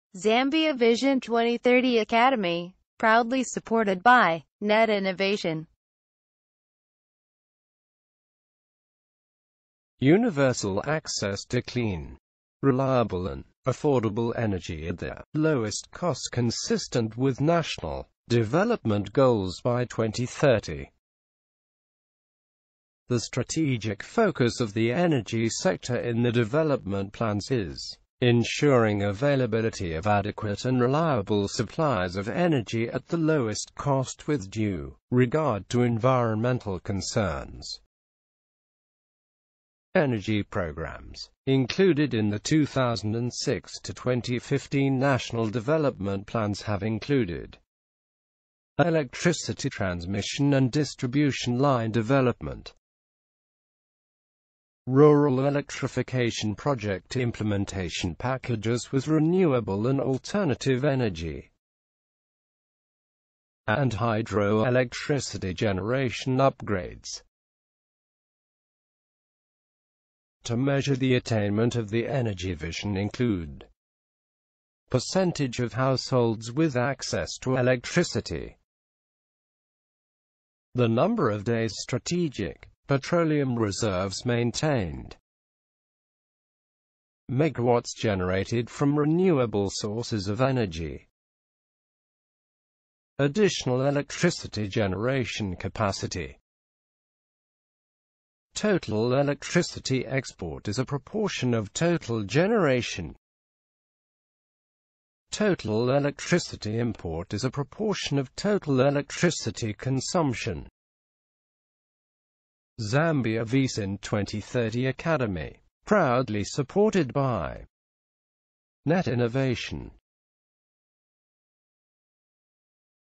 Energy Zambia Vision 2030: Access to clean, reliable and affordable energy by 2030A
Energy Vision for Zambia by 2030

“To have universal access to clean, reliable and affordable energy at the lowest cost consistent with
national development goals by 2030”.

Goal
Ensure that adequate and reliable supply of energy at the lowest cost possible.

Strategic Focus
The strategic focus of the Energy Sector in the R- SNDP will be ensuring availability of adequate and
reliable supplies of energy at the lowest cost with due regard to environmental concerns.

This will be
achieved with due focus on promoting employment creation by the Sector and enhancing rural
development.

Electricity Generation,
Transmission and
distribution Line
Development

Rural Electrification
Project Implementation
Packages

Strategic Petroleum
Reserves

Renewable and
Alternative Energy
Development
Key Perfomance Indicator
Percentage of households
with access to electricity
a) Rural
b) Urban

Strategic Petroleum Reserves
Maintained (No. of Days)

Megawatts generated from
renewable sources of energy

Additional Electricity
generation capacity  (MW)

Energy Trading
a) Total Electricity Export
as a Proportion of total
generation

b) Total Electricity Import
electricity consumption

Take online course and earn certificate.
Zambia Vision 2030 academy proudly supported by Net Innovation